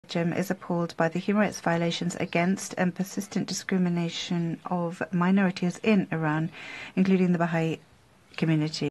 Belgium's statement regarding Baha’is in Iran to the 55th UN Human Rights Council
Belgium spoke at the 55th UN Human Rights Council session, during an interactive dialogue with the UN Special Rapporteur on the human rights situation of the Islamic Republic of Iran, and called on the Iranian government to respect the rights of Iran’s Baha’i religious minority.